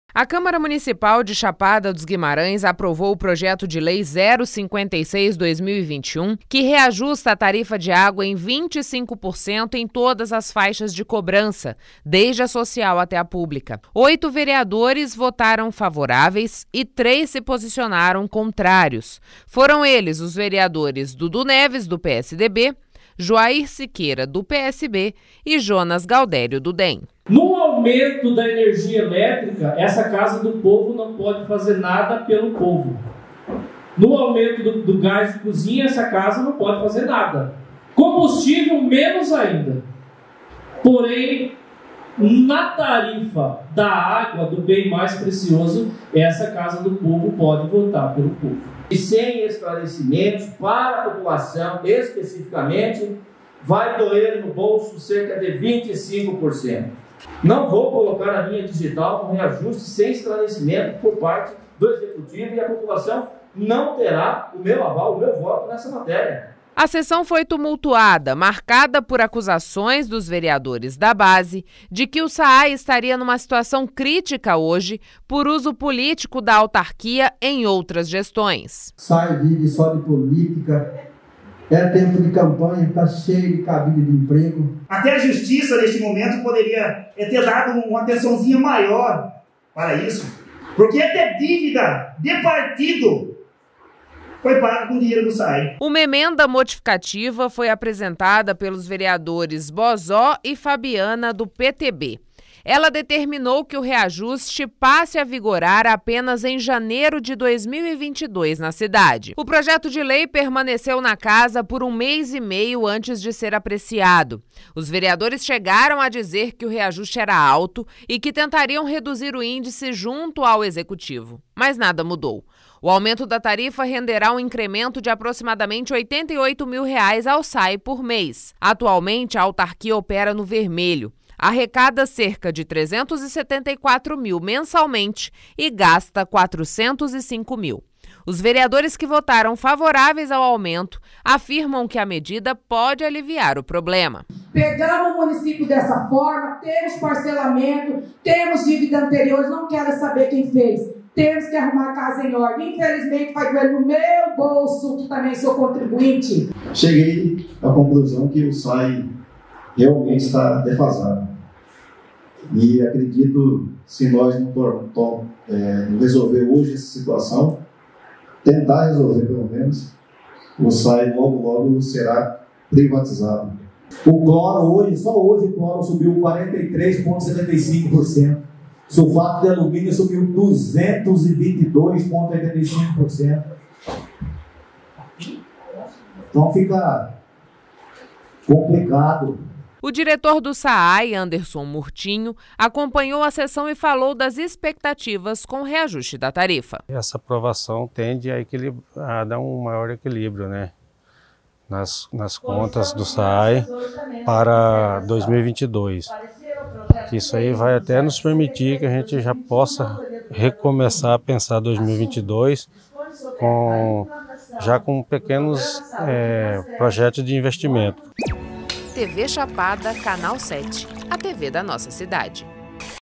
Por 8 votos a 3, Câmara aprova reajuste de 25% na tarifa de água
Um mês e meio após receber o projeto de lei 056/2021, que prevê o reajuste da tarifa de água em cerca de 25%, a Câmara Municipal aprovou a proposta encaminhada pelo Executivo com 8 votos favoráveis e 3 contrários. O aumento passa a vigorar a partir de janeiro de 2022 e deve garantir ao SAAE um incremento de R$ 88 mil.